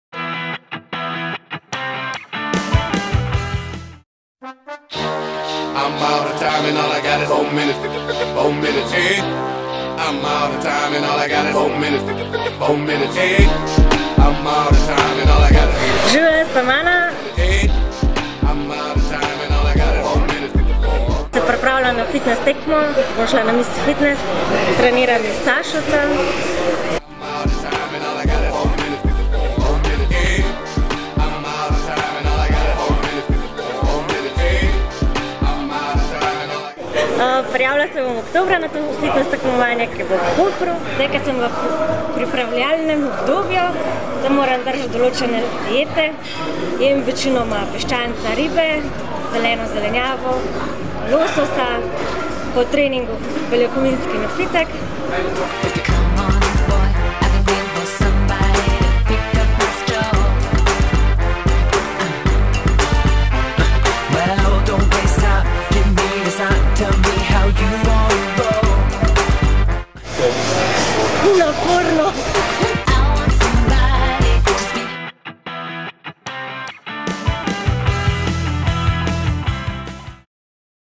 Ana Poljanšek pripava na Miss Fitness
1. del priprav na tekmo za miss fitness s trenerjem Sašom Petkom. Spremljajte preobrazbo...